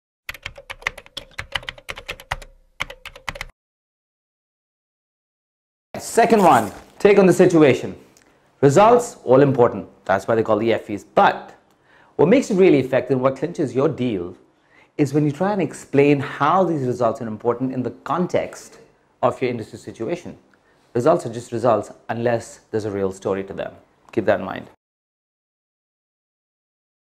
The A to Z on how to win The Effies - EP 7
Get to know the A to Z of how to win an Effie - presenting the seventh episode of the series featuring Mr. Aly Mustansir.